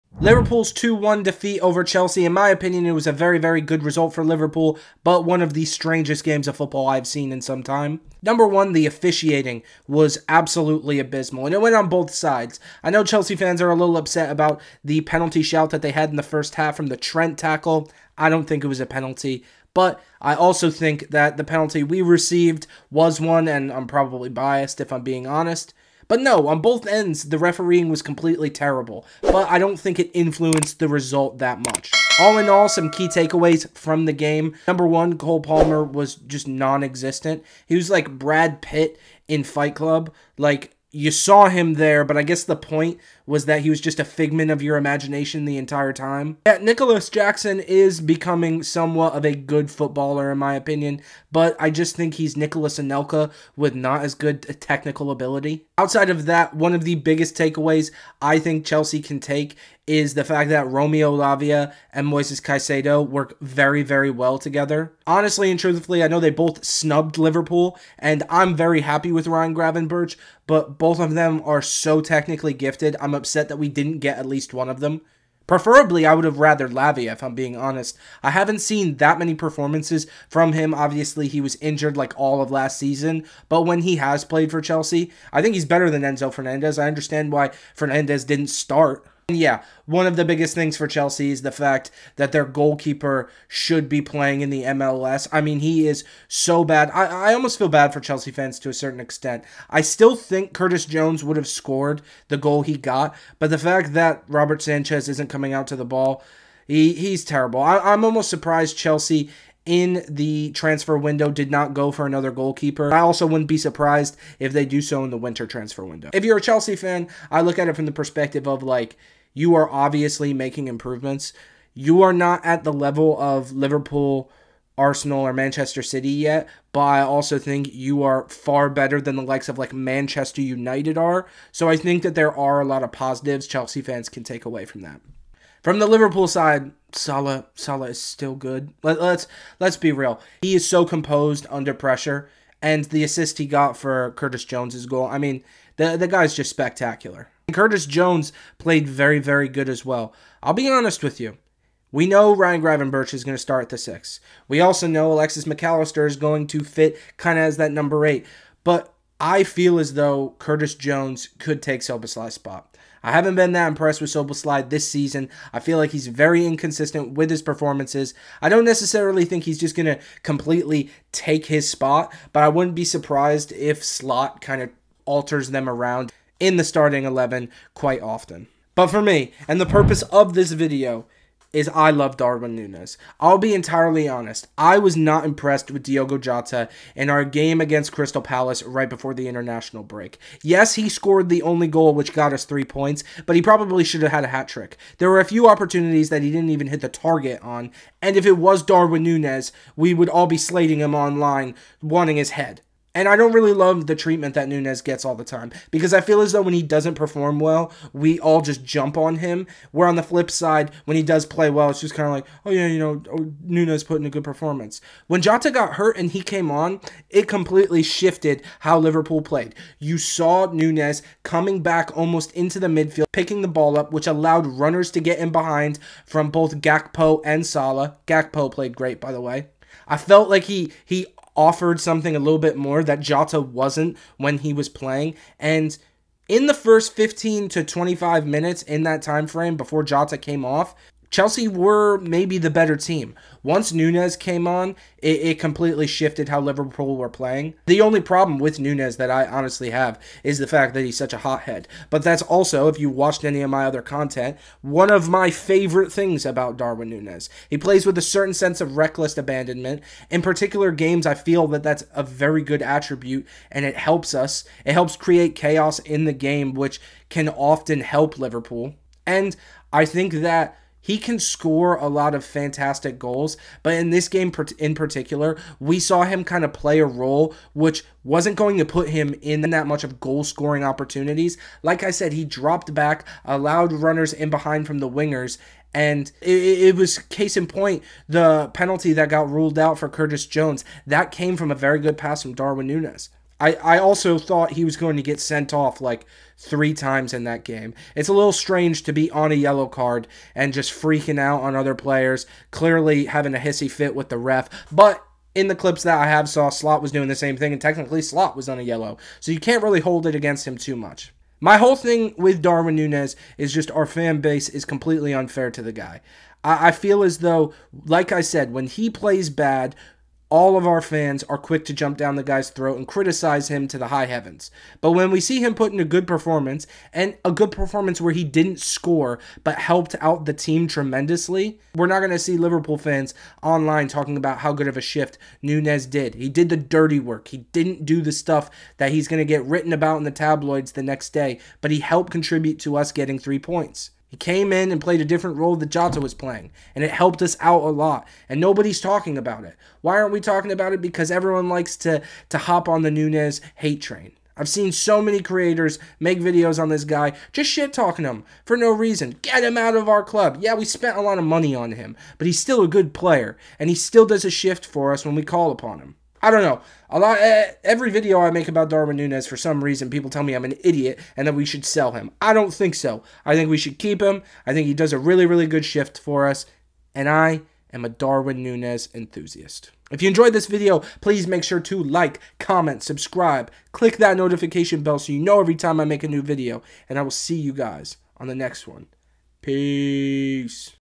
Darwin Nunez Should START for Liverpool!
In this video I break down why I believe Darwin Nunez should be a regular starter for Liverpool!

Follow me on X: LeemoLiverpool